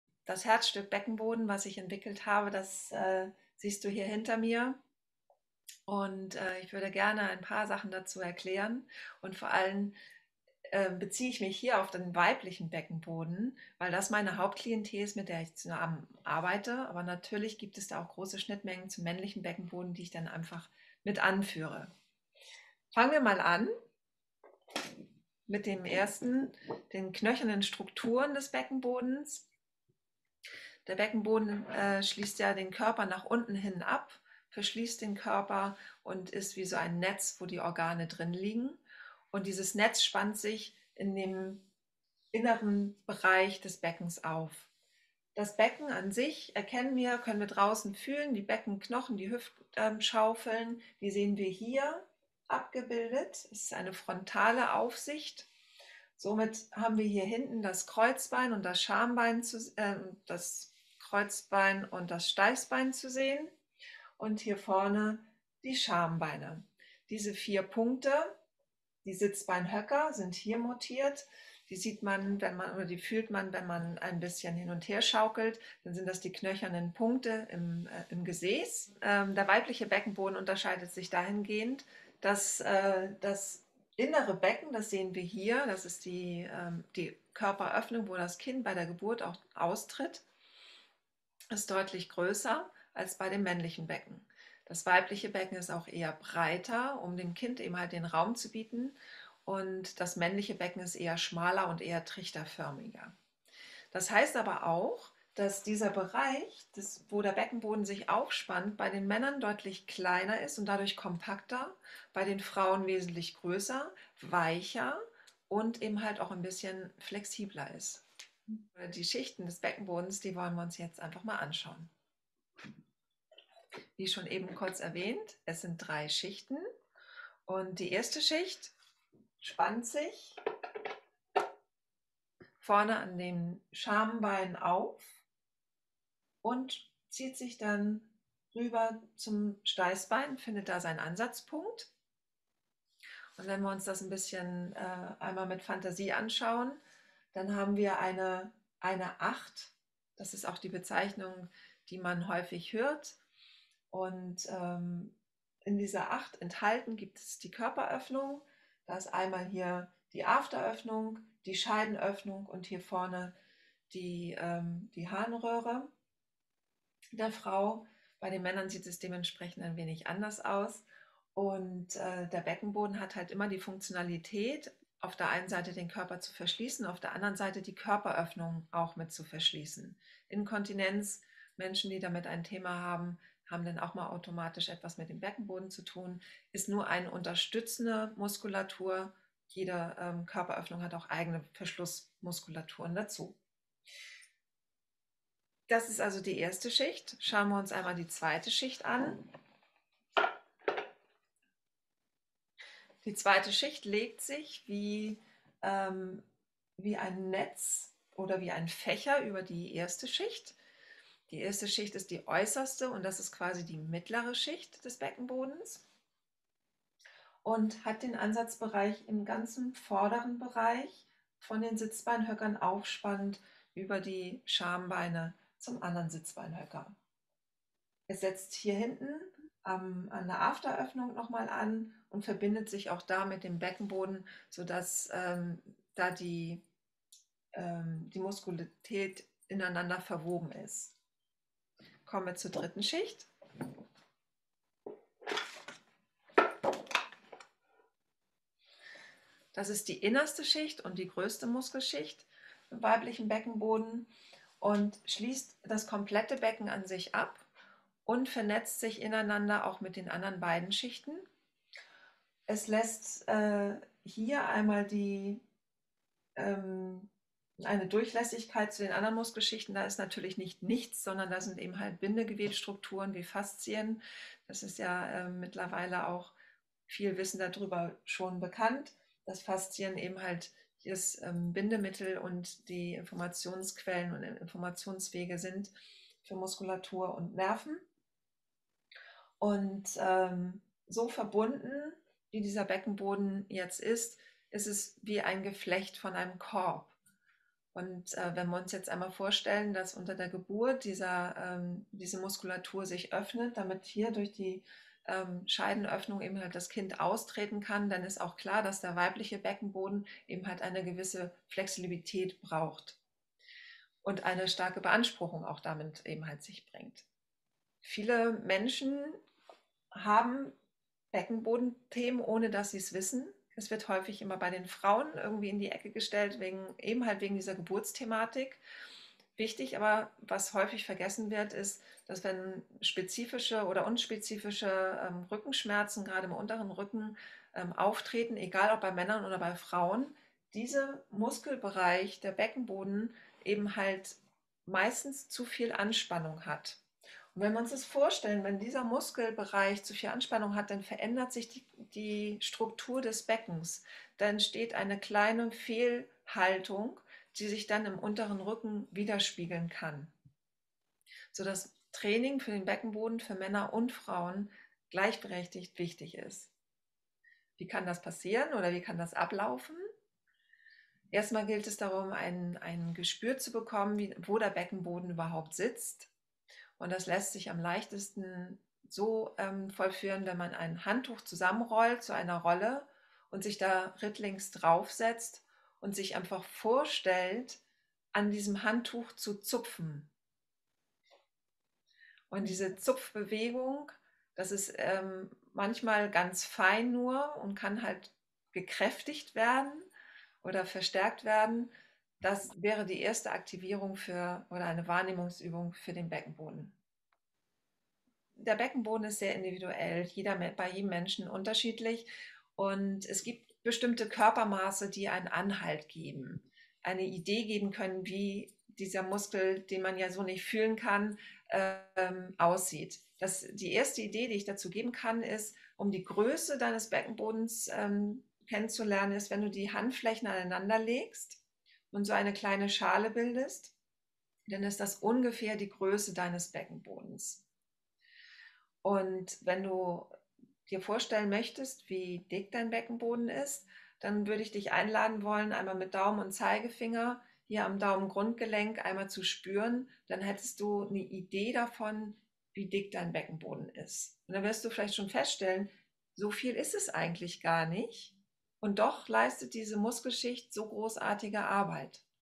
TOK-Movie Clip: Der Beckenboden - Grundlagenwissen
Gesa Ott, Beckenbodentrainerin und Shiatsutherapeutin hat ein sehr anschauliches Modell des Beckenbodens entwickelt und erklärt anhand dessen den Aufbau des Beckenbodens sehr anschaulich. Das Modell kann bei ihr käuflich erworben werden.

Mehr zum  Beckenboden und wie Du ihn mit Taijiquan und Qigong stärken kannst, in der Sendung von Treasures and knowledge Sendung "he pelvic floor - source of energy and misunderstanding"